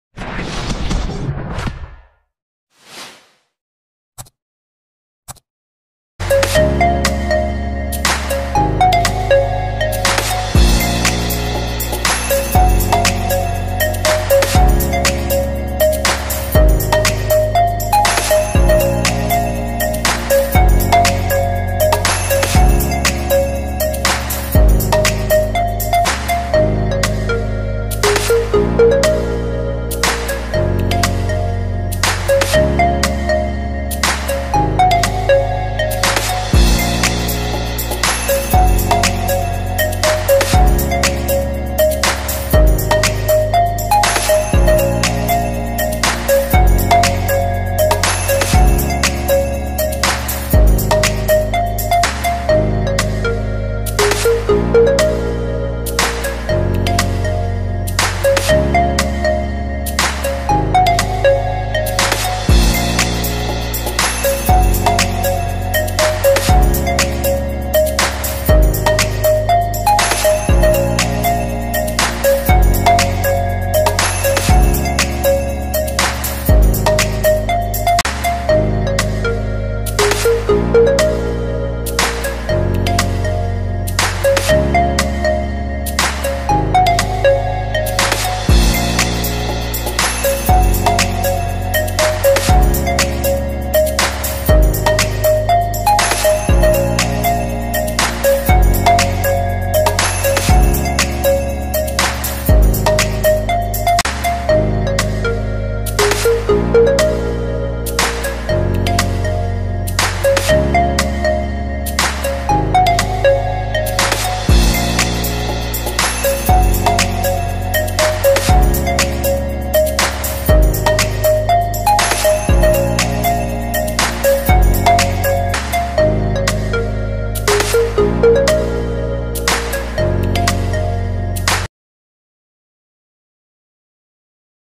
FC Barcelona's all time great goals/FiFa Worldcup Fever
This video is about the FC Barcelona's best goals of all times from their great players.